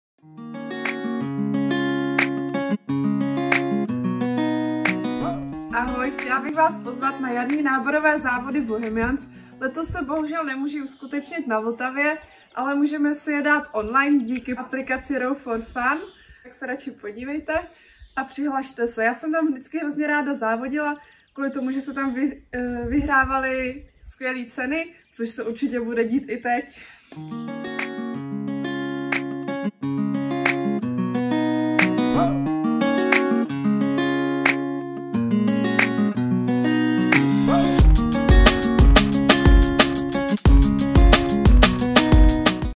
Regata Bohemians - virtuální závod na Row4fun.com
Kristýna Fleissnerová zve na Regatu Bohemians online :)

A pozor! Přihlášení posádek (2x a smíšená 4x) je do 17. dubna.